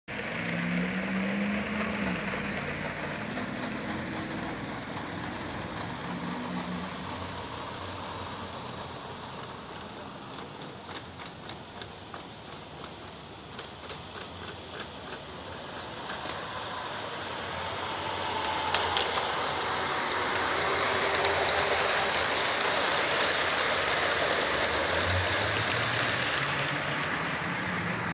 H & R Trains G Scale Trains
G Scale trains running on the outdoor layout.